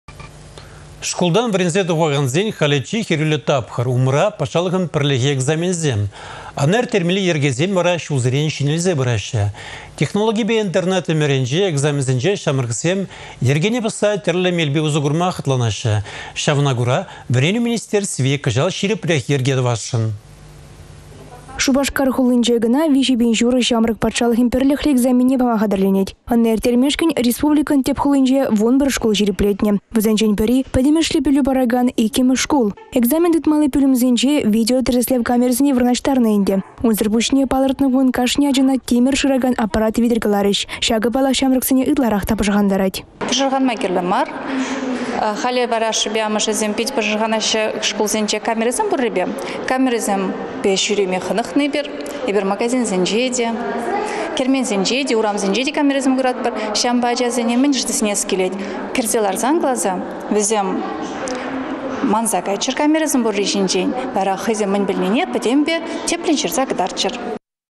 Шупашкар ачисем экзаменсене хатĕрленеççĕ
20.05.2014
Шкултан вĕренсе тухакансен халĕ чи хĕрÿллĕ тапхăр. Умра - патшалăхăн пĕрлехи экзаменĕсем. Ăна ирттермелли йĕркесем вара çулсерен çĕнелсе пыраççĕ. Технологипе интернет ĕмĕрĕнче экзаменсенче çамрăксем йĕркене пăсса тĕрлĕ мелпе усă курма хăтланаççĕ. Çавна кура вĕренÿ министерстви кăçал çирĕпрех йĕрке тăвасшăн.